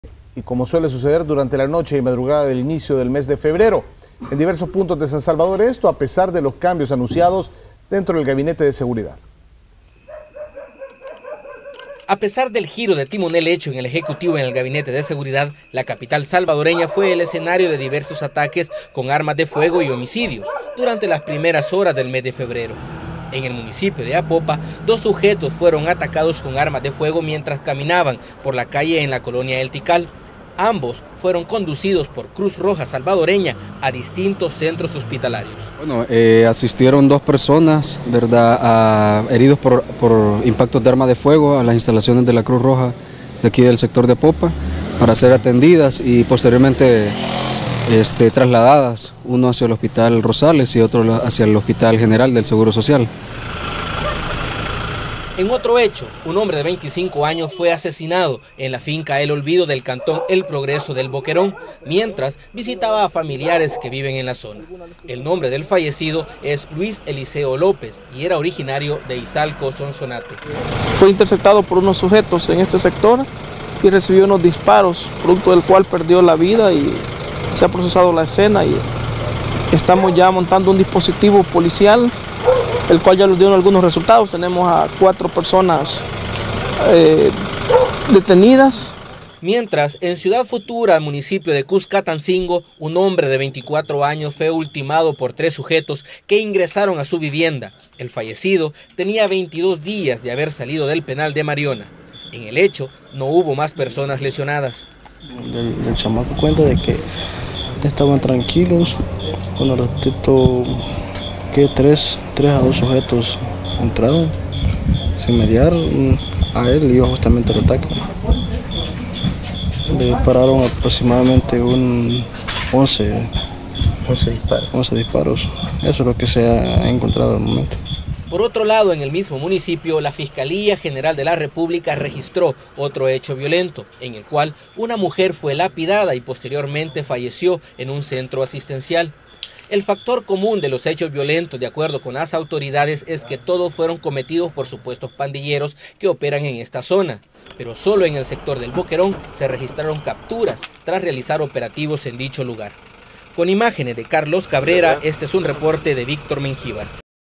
Diversos crímenes fueron registrados en la capital del país
A pesar de las modificaciones en el gabinete de seguridad, varios crímenes se registraron en diversas localidades del país.